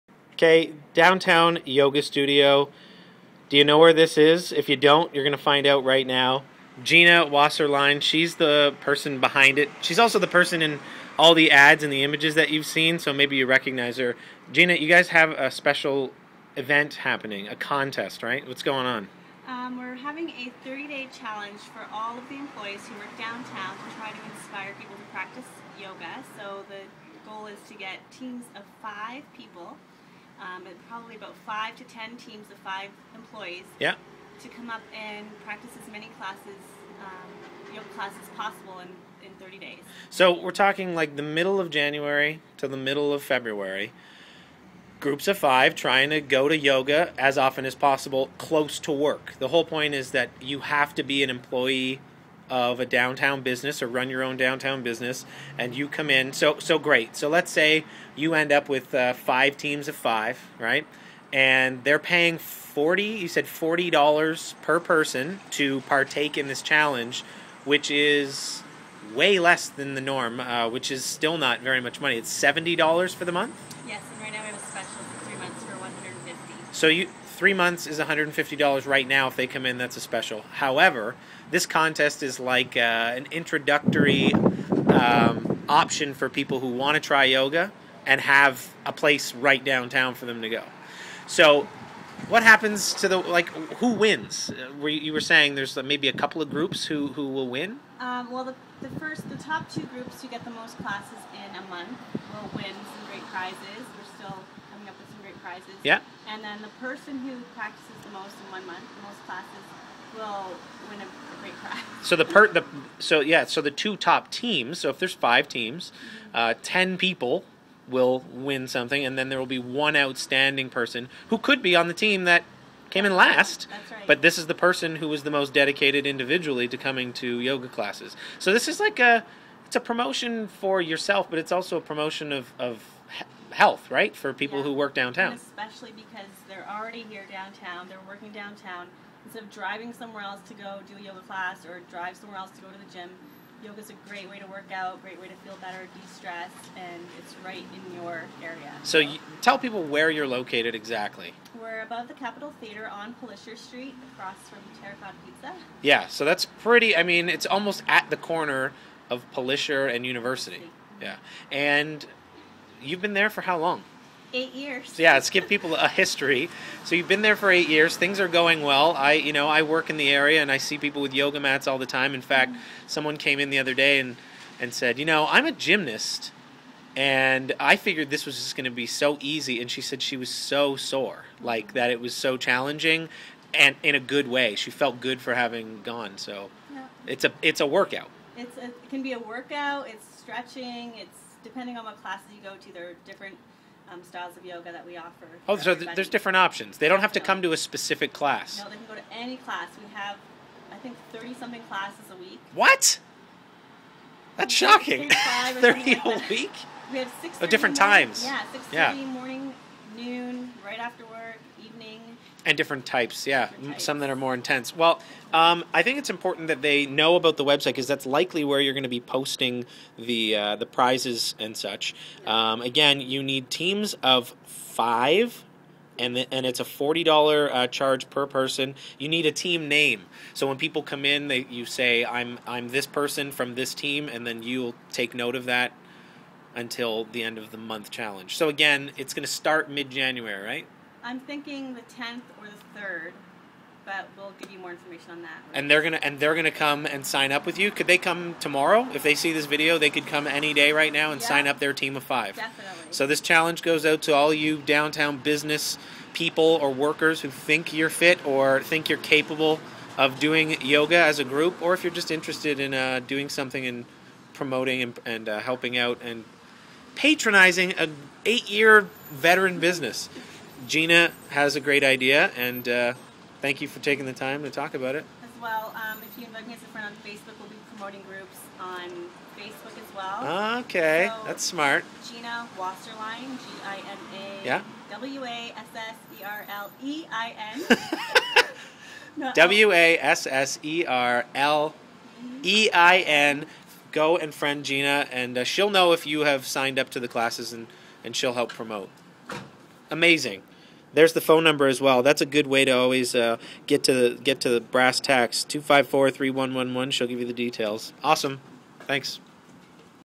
Downtown Yoga Studio Challenge Interview
Downtown Yoga Studio Challenge Interview with Gina Wasserlein.

Filmed by Tom Lucier on December 10, 2010.